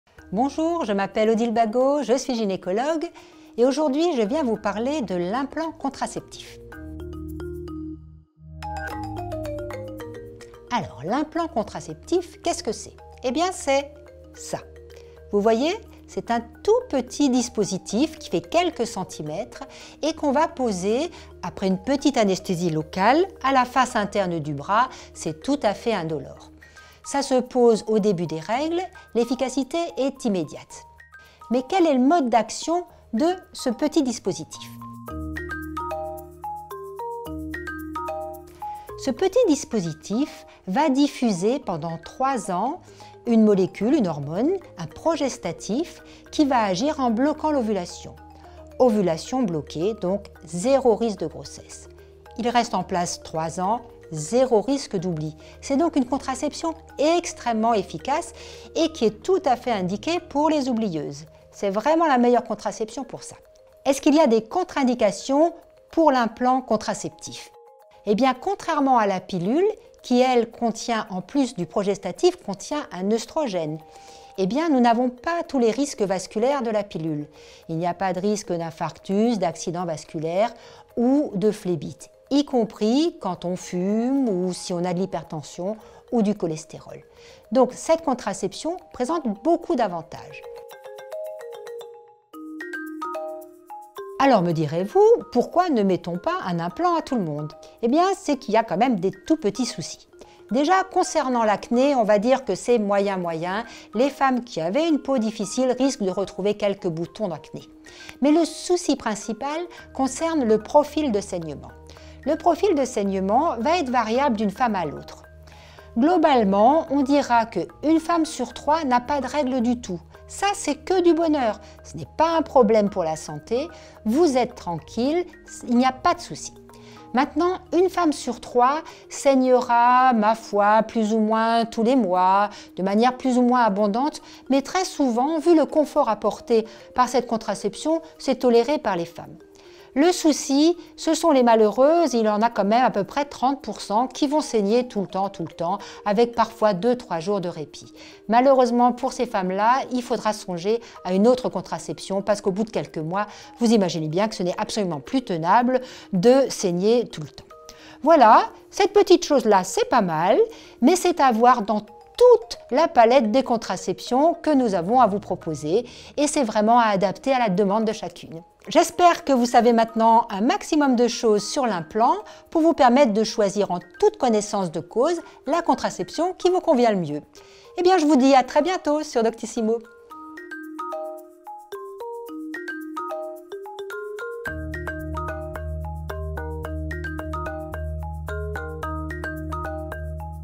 Implant contraceptif : avantages et inconvénients - Gynécologie
L’implant contraceptif est un petit bâtonnet en plastique, très fin, qu’on insère sous la peau. Il délivre une progestérone de synthèse contraceptive. Qui peut utiliser cette contraception ? Quels sont les avantages et les inconvénients ? Le point avec Odile Bagot, gynécologue.

Production : LED